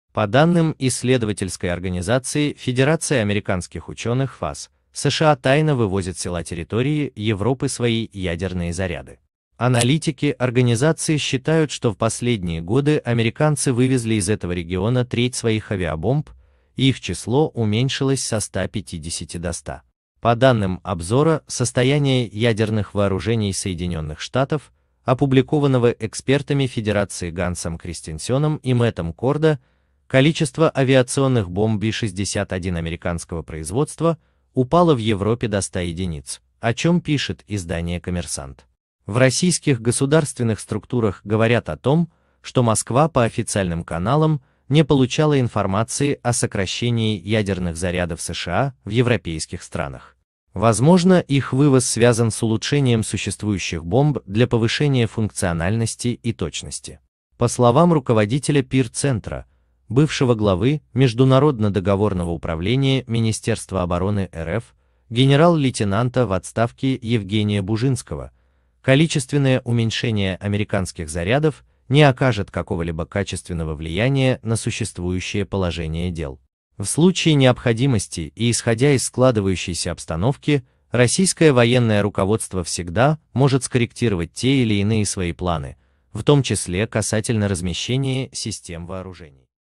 СМИ сообщили о тайном вывозе американских ядерных зарядов из Европы
По данным исследовательской организации «Федерация американских ученых» (FAS), США тайно вывозят с территории Европы свои ядерные заряды. Аналитики организации считают, что в последние годы американцы вывезли из этого региона треть своих авиабомб, и их число уменьшилось со 150 до 100.

По данным обзора состояния ядерных вооружений Соединенных Штатов, опубликованного экспертами Федерации Гансом Крестенсеном и Мэттом Корда, количество авиационных бомб B 61 американского производства упало в Европе до ста единиц, о чем пишет издание «Коммерсант».

В российских государственных структурах говорят о том, что Москва по официальным каналам не получала информации о сокращении ядерных зарядов США в европейских странах. Возможно, их вывоз связан с улучшением существующих бомб для повышения функциональности и точности.

По словам руководителя ПИР-Центра, бывшего главы международно-договорного управления Министерства обороны РФ, генерал-лейтенанта в отставке Евгения Бужинского, количественное уменьшения американских зарядов не окажет какого-либо качественного влияния на существующее положение дел. В случае необходимости и исходя из складывающейся обстановки российское военное руководство всегда может скорректировать те или иные свои планы, в том числе касательно размещения систем вооружений.